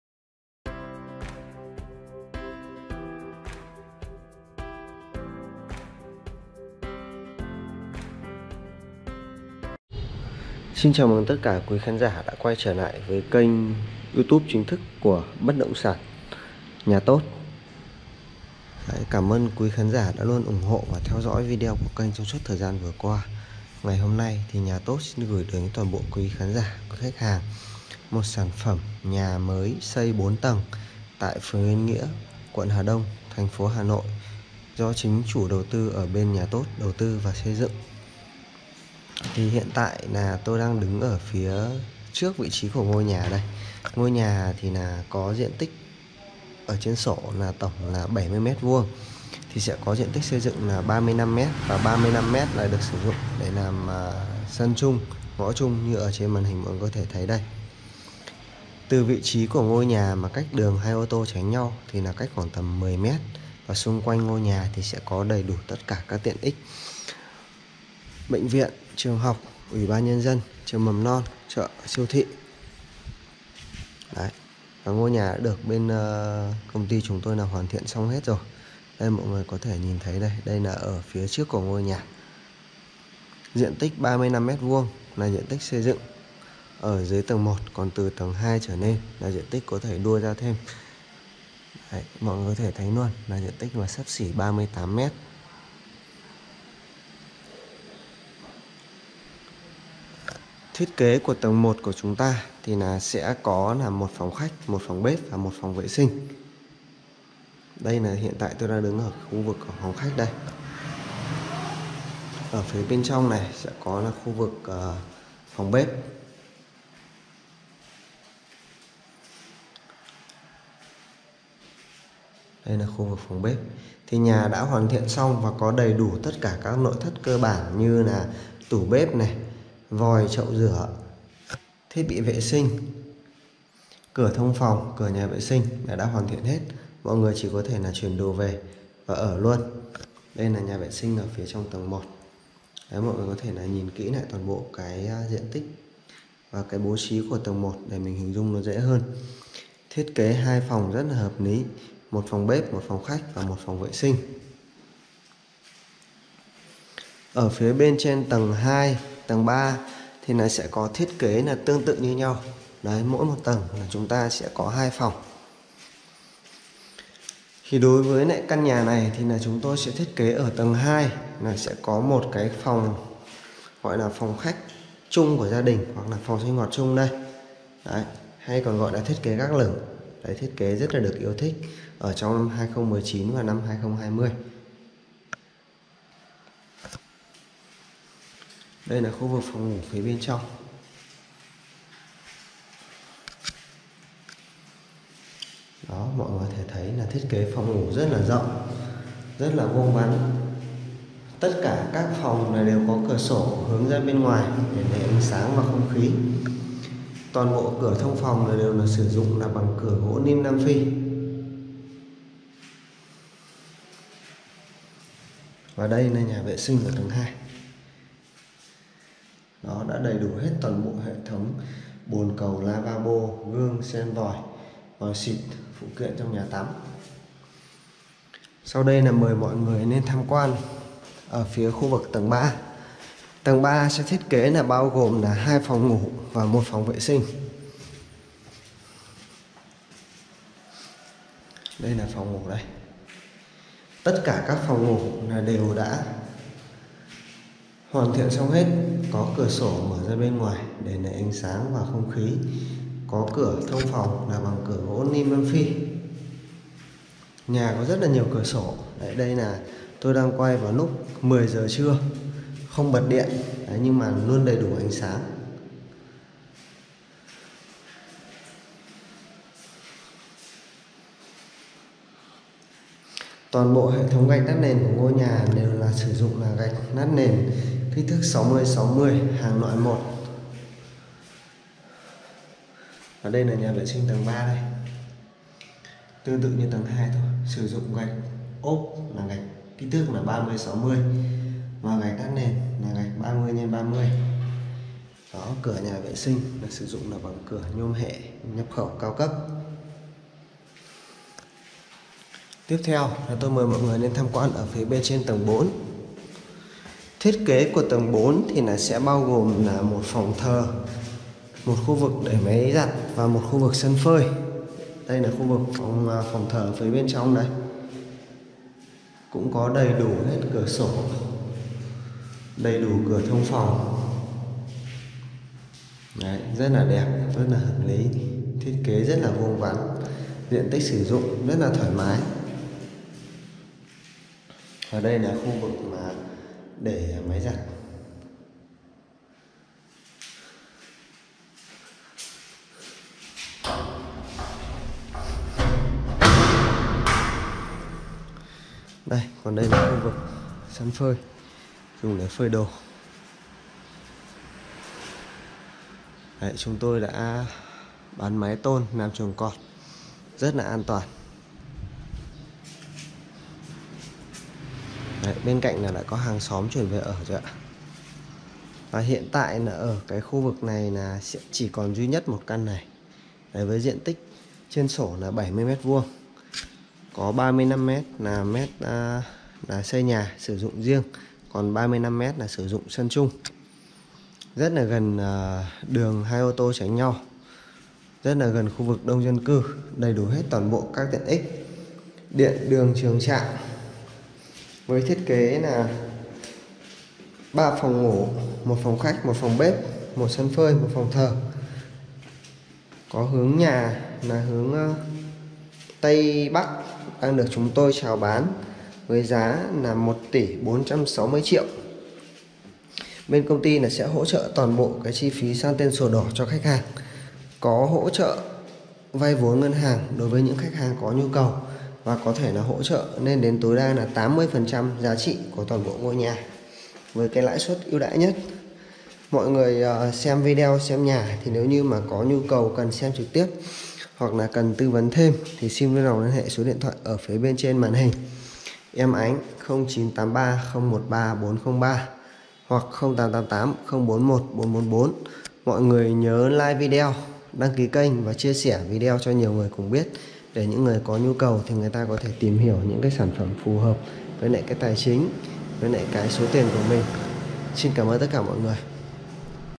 Bán nhà Hà Đông xây mới 4 tầng, 35m2 giá rẻ, sổ đỏ chính chủ | nhà TỐT
Bán nhà Hà Đông xây mới 4 tầng, 35m2 giá rẻ, sổ đỏ chính chủ
- Địa chỉ: tổ 14, phường Yên Nghĩa, quận Hà Đông, Hà Nội
- Diện tích: 35m2
- Hướng nhà: Tây Nam
- Thiết kế: 4 tầng, 3 phòng ngủ, 1 phòng khách, 1 phòng bếp, 1 phòng thờ, 3WC